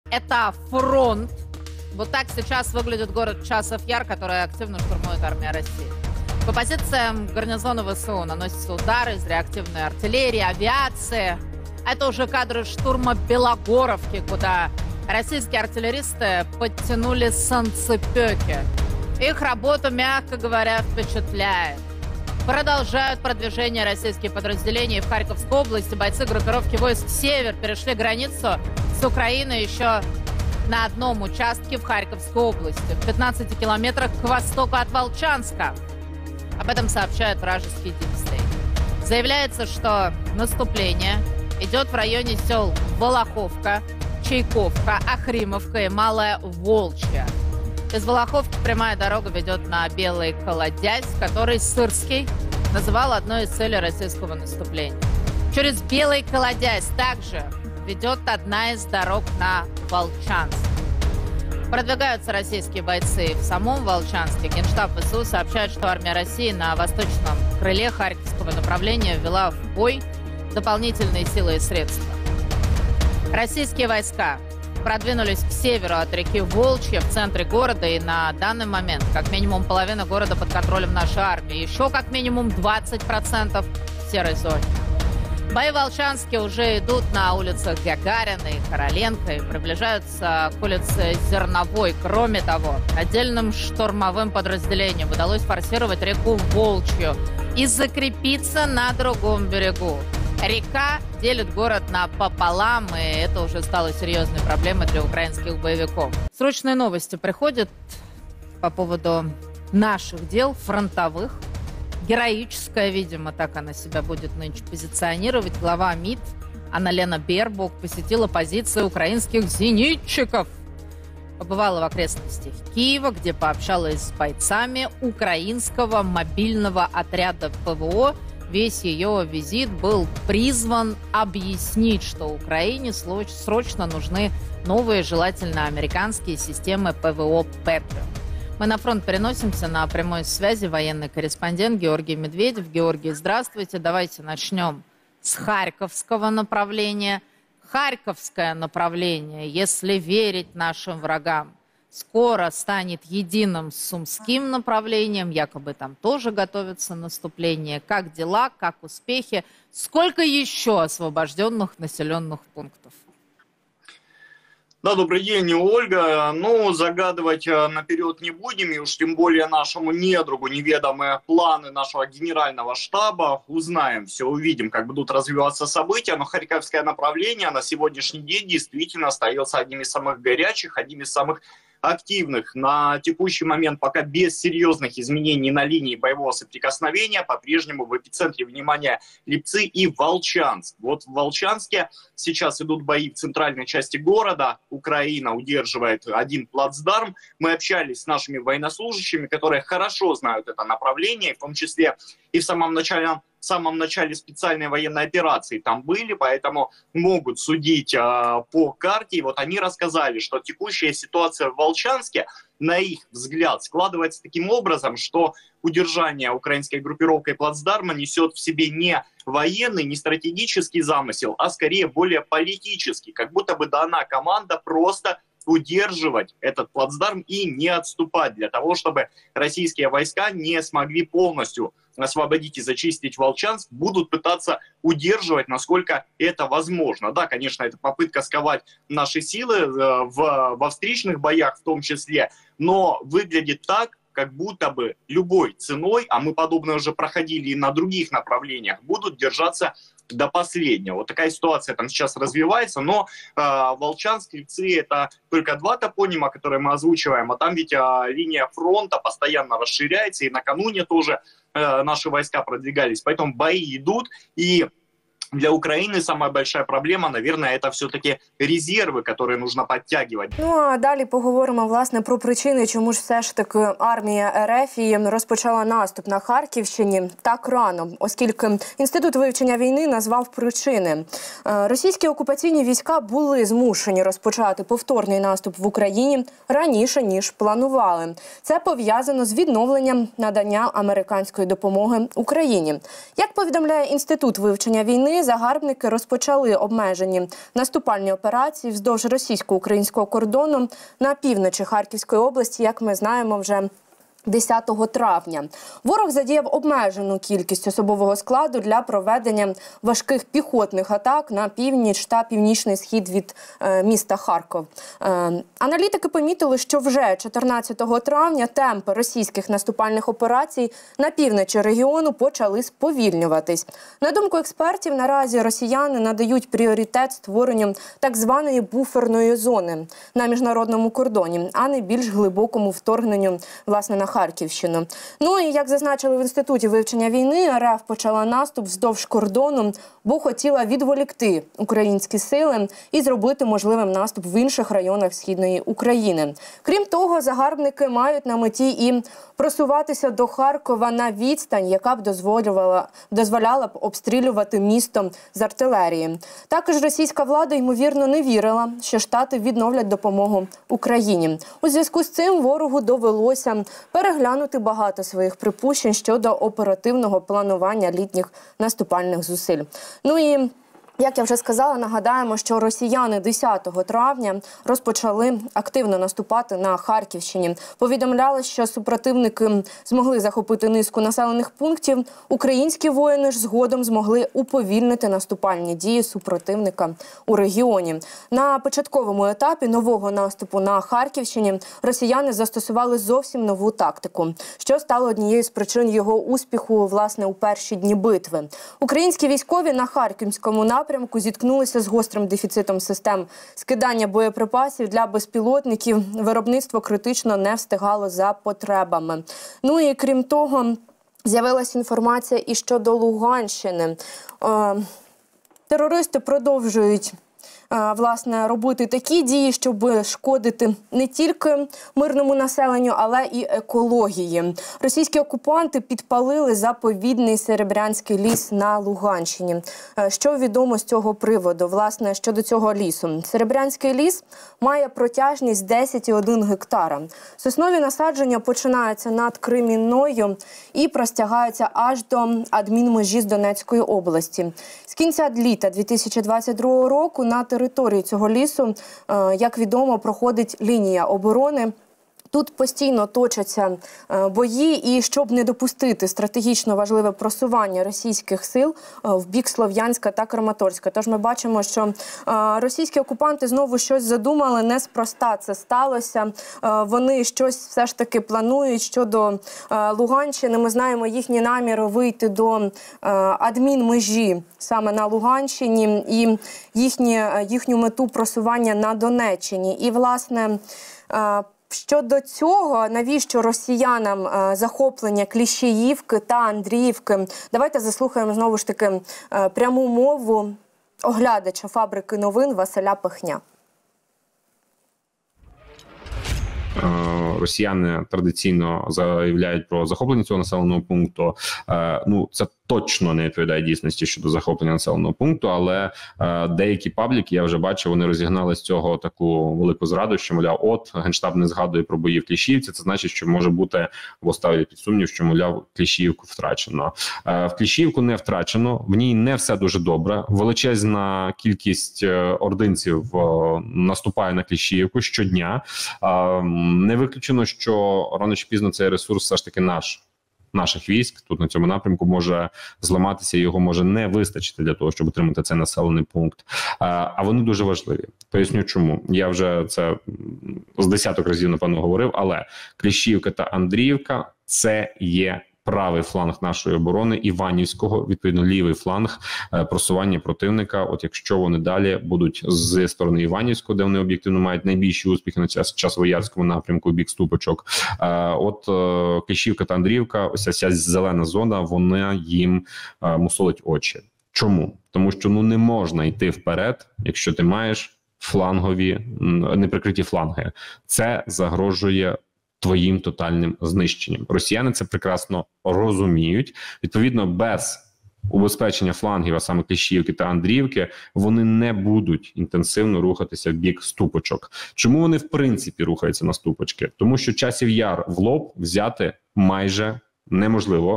Під час ефіру Скабєєвої біля студії у Москві ПОЧУЛИ ВИБУХ! Всі перелякалися, почалася ПАНІКА?!
В ефірі Скабєєвої почалася паніка через звуки вибухів у Москві?!